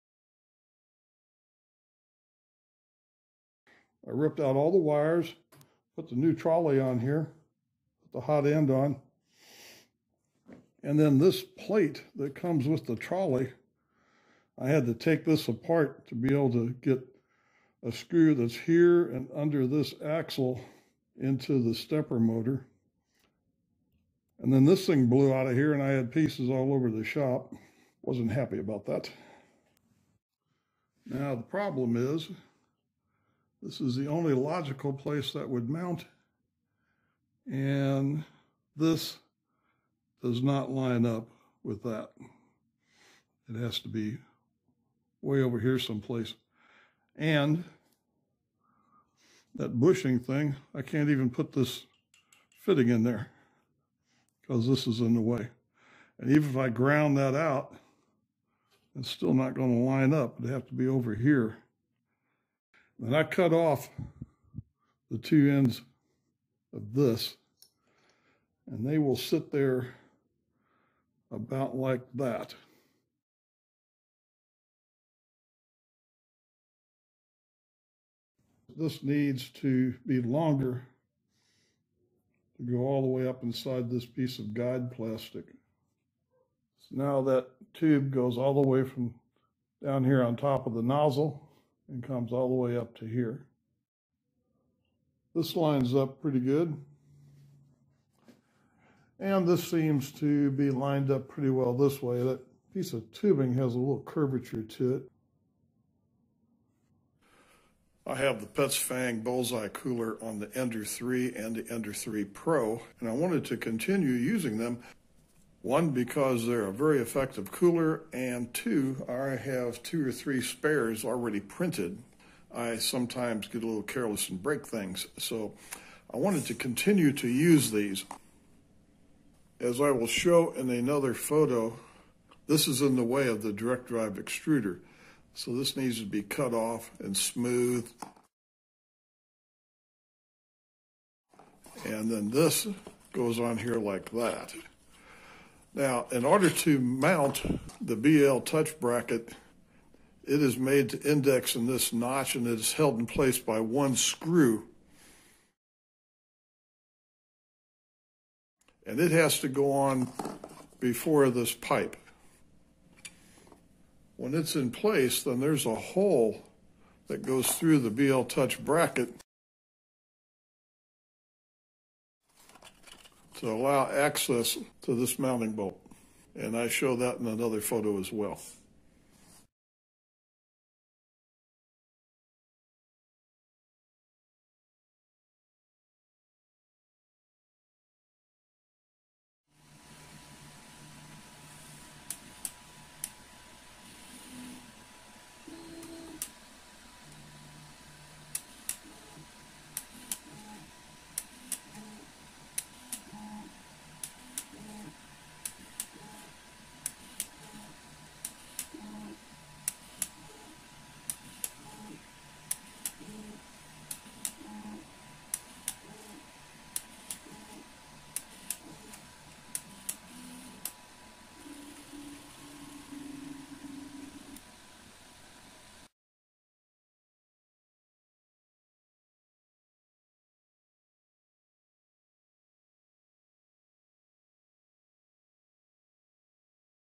Ender-3 Pro Upgrade - Direct Drive Extruder & BLTouch
Ender-3 Pro upgrade for direct drive extruder and BLTouch auto leveling, a new trolley was purchased with an extruder mounting bracket.

I also purchased a Befenybay direct drive extruder, which came with a shorter (lighter) stepper motor.

The original hot end (which came with the Ender-3 Pro) was reused.

The extruder mounting bracket needed quite a bit of modification to allow correct alignment of the extruder with the hot end.

After cutting away pieces of the extruder mounting bracket, new holes were drilled in the trolley bracket. I attached the two with bolts and nuts.

Adding the Sovol Creality Ender 3 Pro 32-bit Silent Board Motherboard V4.2.7 with TMC2225 Driver Marlin 2.0.1 for Ender-3 Pro really makes it quiet and provides connections for the BLTouch.

UniTak3D Ender 3 Direct Drive Upgrade Kit Conversion & Bracket

Befenybay Upgraded 3D Printer Extruder Parts with Stepper Motor for ANYCUBIC Mega-SCR10V6 Hotend J-Head for 1.75mm Filament (Extruder with Stepper Motor)

Automatic Bed Leveling Sensor 3D Touch Auto Leveling for 3D Printers Ender-3 Pro

Sovol Creality Ender 3 Pro 32-bit Silent Board Motherboard V4.2.7 with TMC2225 Driver Marlin 2.0.1 for Ender-3 Pro